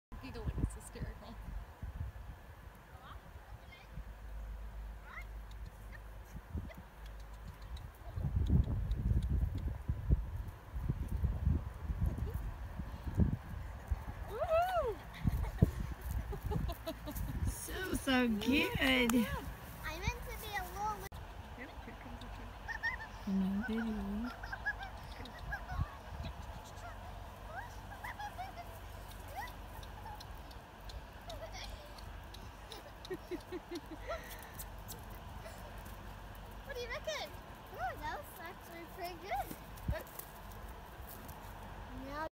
Riding Joker
Riding Joker (Joka) in the back yard